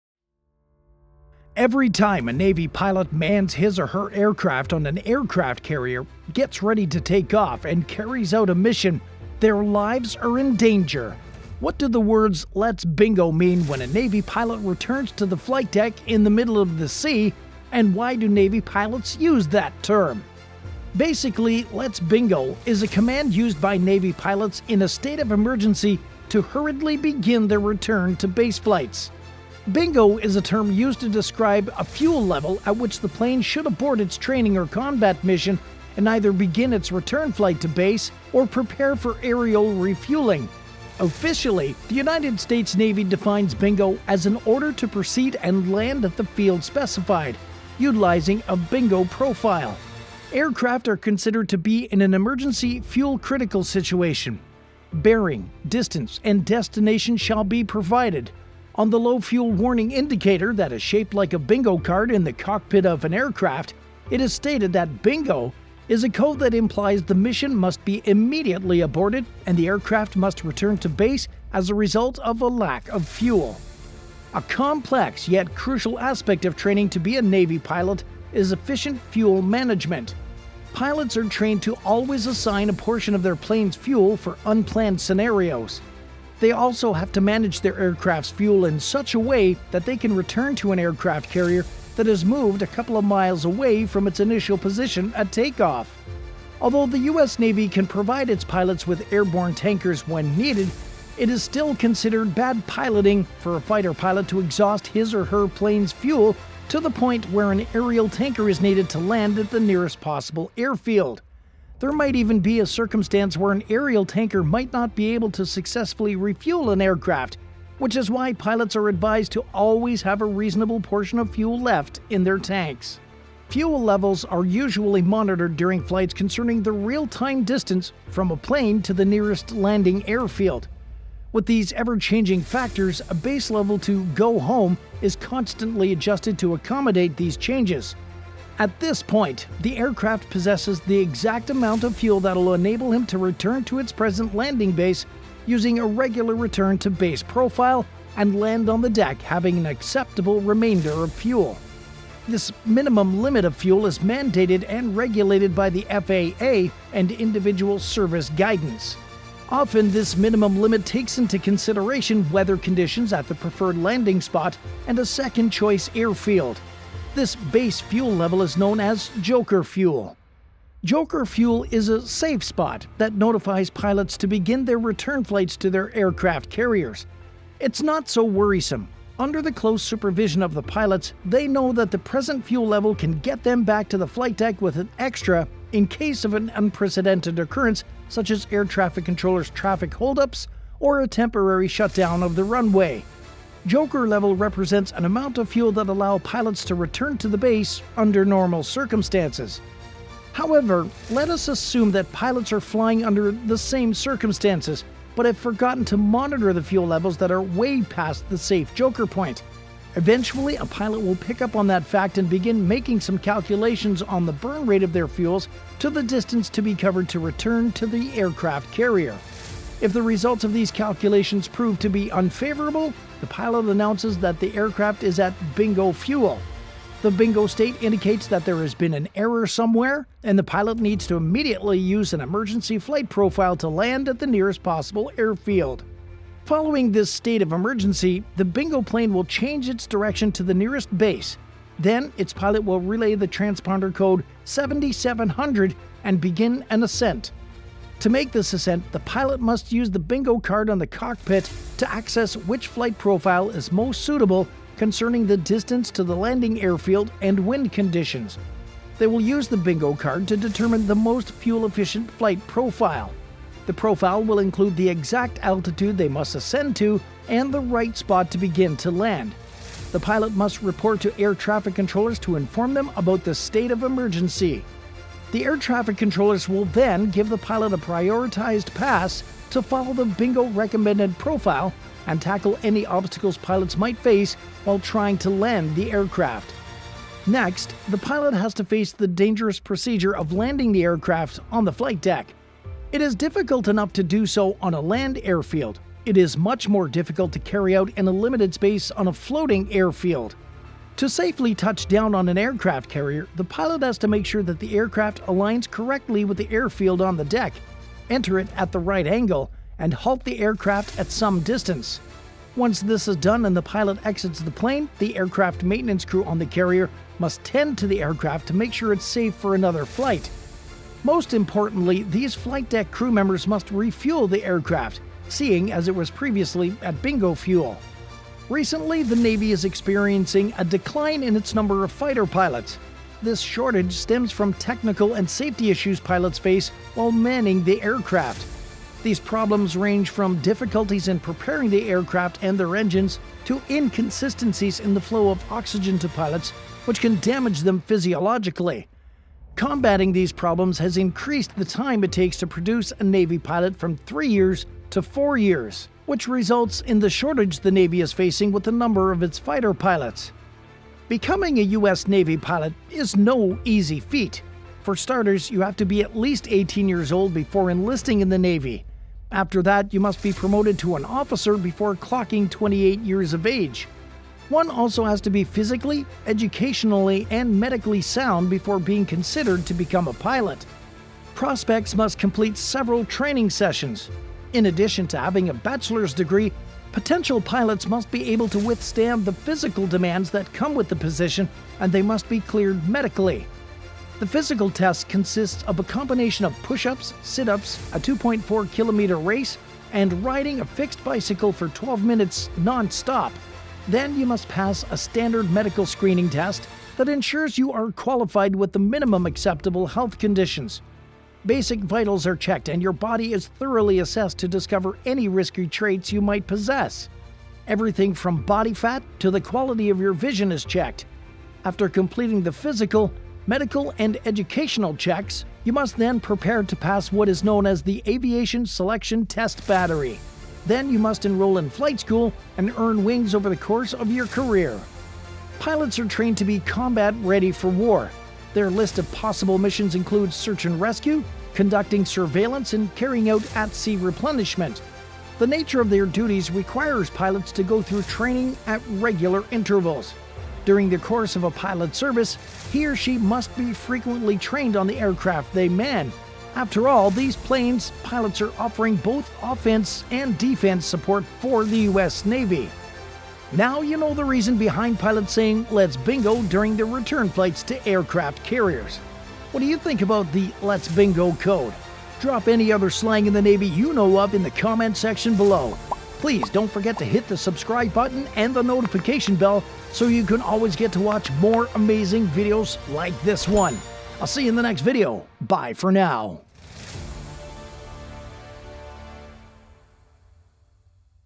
Why Pilots Says “Let’s Bingo” During Flight to an Aircraft Carrier
Every time a US navy pilot mans his or her aircraft on an aircraft carrier, gets ready to take off, and carries out a mission, their lives are in danger. What do the words let's bingo mean when a us navy F18 pilot returns to the flight deck in the middle of the sea? and why do Navy pilots use it? Air to Air refueling, F-18 Pilot Aerial  refueling.
Basically, "Let's Bingo" is a command used by Navy pilots in a state of emergency to hurriedly begin their return to base flights.
"Bingo" is a term used to describe a fuel level at which the plane should abort its training or combat mission and either begin its return flight to base or prepare for aerial refueling. Officially, the United States Navy defines "Bingo" as "an order to proceed and land at the field specified, utilizing a bingo profile. Aircraft fighter jets are considered to be in an emergency/fuel-critical situation. Bearing, distance, and destination shall be provided." On the low-fuel warning indicator that is shaped like a Bingo card on the cockpit of an aircraft, it is stated that "Bingo" is a code that implies the mission must be immediately aborted and the aircraft must return to base as a result of a lack of fuel.
A complex yet crucial aspect of training to be a Navy pilot is efficient fuel management. Pilots are trained to always assign a portion of their plane's fuel for unplanned scenarios. They also have to manage their aircraft's fuel in such a way that they can return to an aircraft carrier that has moved a couple of miles away from its initial position at take-off. Although the U.S. Navy can provide its pilots with airborne tankers when needed, it is still considered bad piloting for a fighter pilot to exhaust his or her plane's fuel to the point where an aerial tanker is needed to land at the nearest possible airfield. There might even be a circumstance where an aerial tanker might not be able to successfully refuel an aircraft, which is why pilots are advised to always have a reasonable portion of fuel left in their tanks.